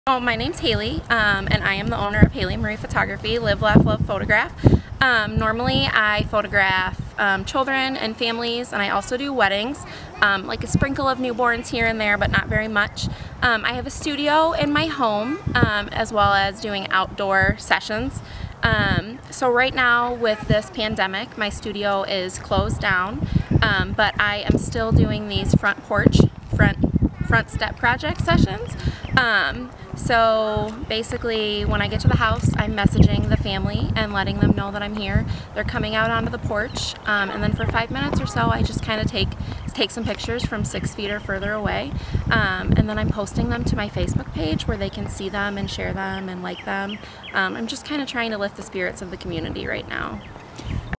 Socially distant photo sessions
Haley Kolle, owner of Haley Marie Photography, describes her business and explains how she has adapted her photo sessions to comply with social distancing rules.